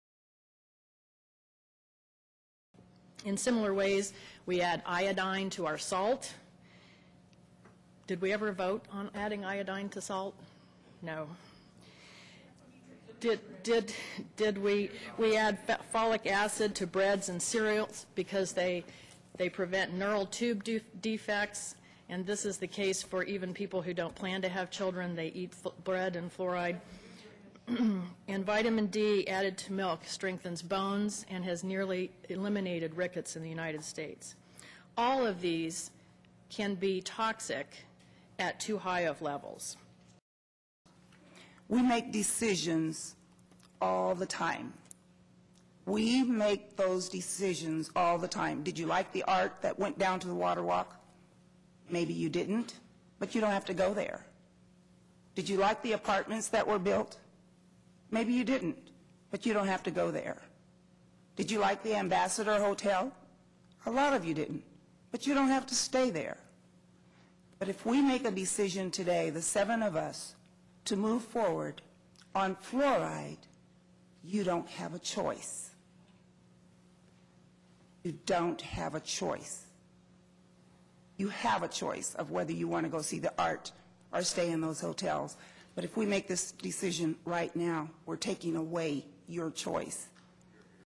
Wichita Fluoridation Debate Reveals Attitudes
Remarks by Wichita City Council Members Janet Miller and Lavonta Williams reveal a troubling attitude towards the role of government and its relationship with citizens.